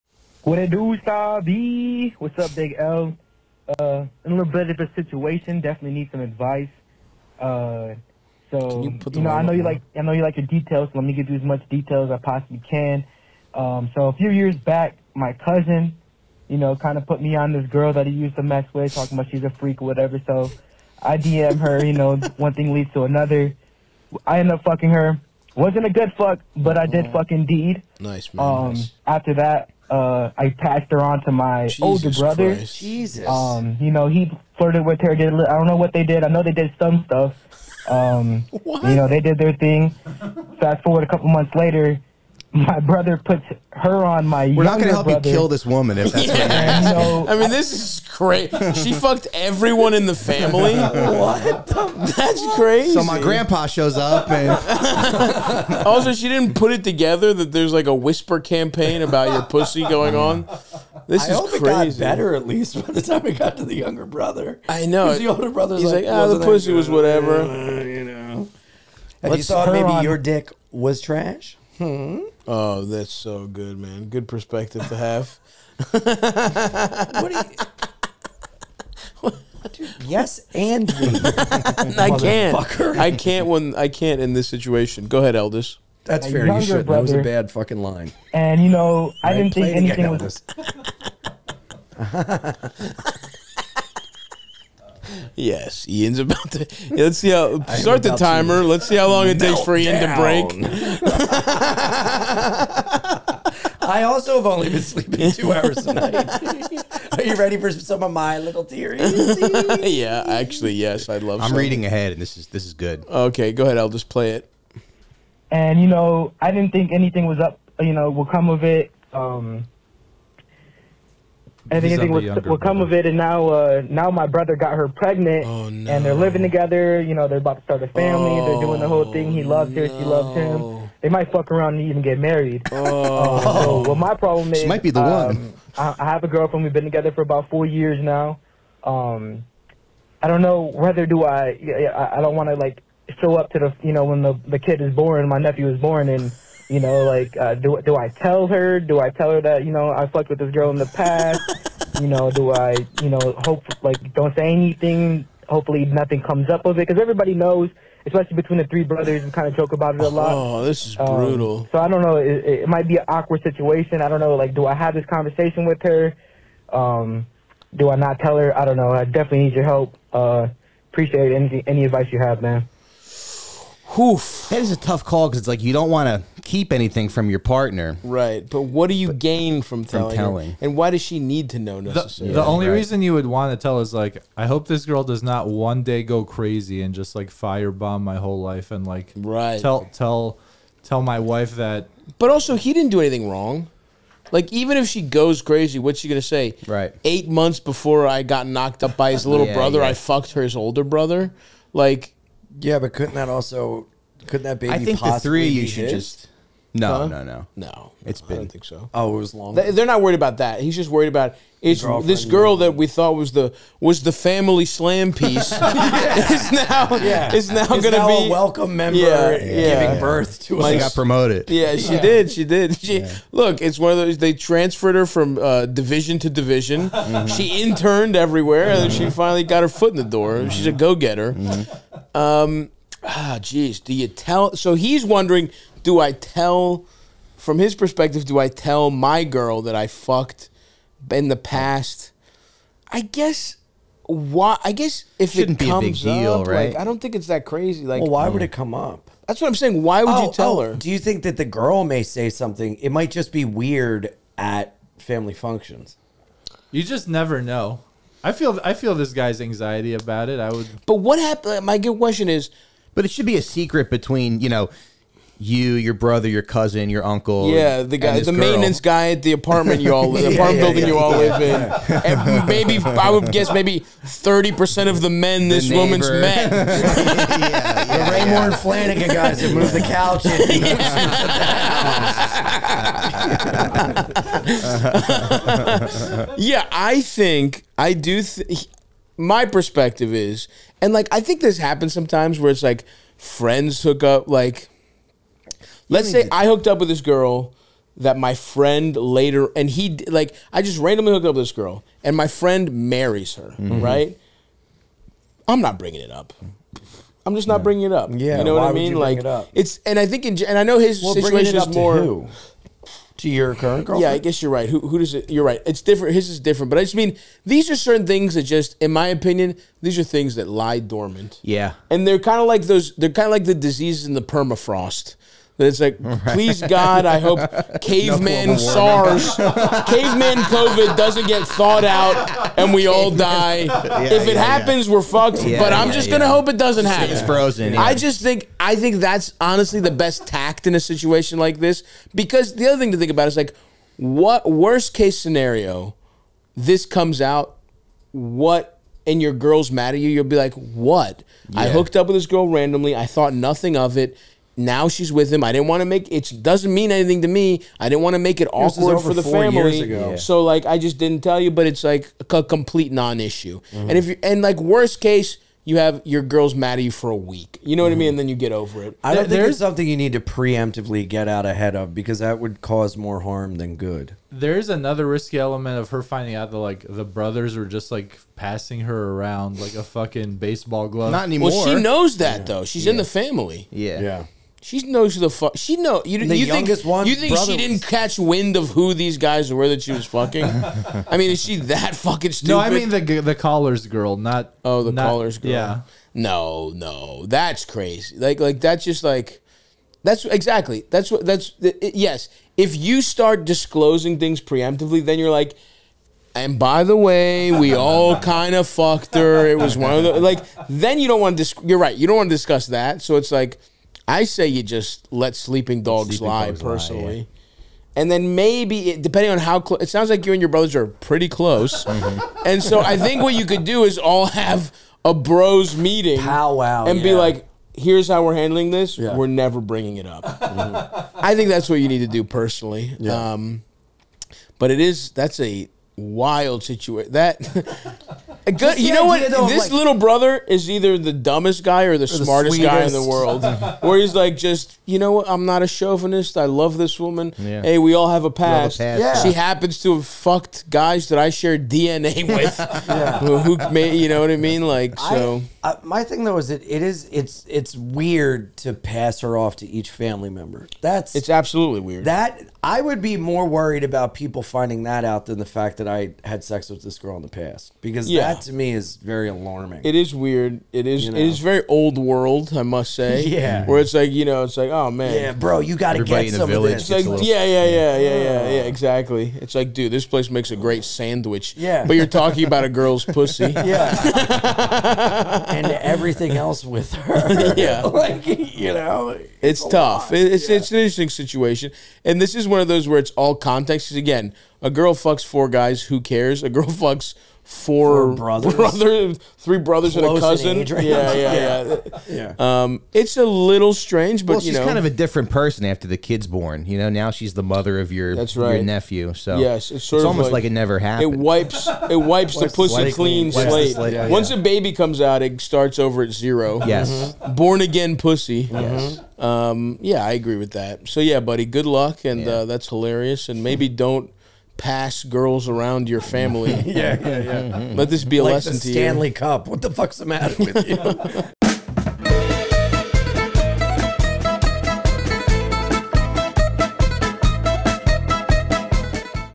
I banged my brother's baby mama; should I tell my girlfriend?
Friends of the pod Ian Fidance and Mike Recine return to celebrate Greek Independence Day and discuss bathroom etiquette, famous Greek figures in history, the dastardly Ottomans, Teenage Mutant Ninja Turtles, Mike's tomato sauce, Bernie Mac, Eddie Griffin, Google results for their names, and much more. Ian, Mike, and Stav help callers including a guy who has a history with his brother's baby mama, and a sperm donor who's wondering if he should tell his parents he's donating to a friend.

Watch Ian Fidance special 'Wild, Happy and Free' coming out on 4/4!